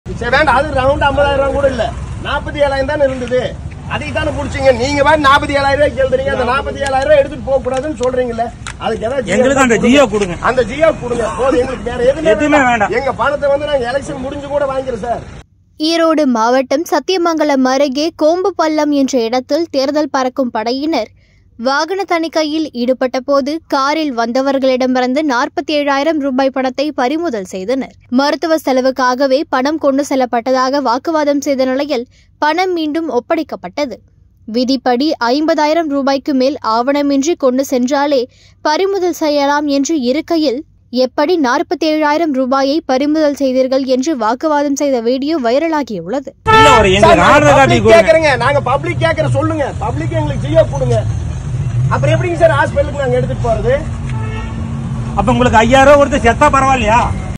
47,000 ரூபாய் கொண்டு போகக் கூடாதா? கடுப்பான இளைஞர்
47,000 ரூபாய் கொண்டு போகக் கூடாதா? கடுப்பான இளைஞர்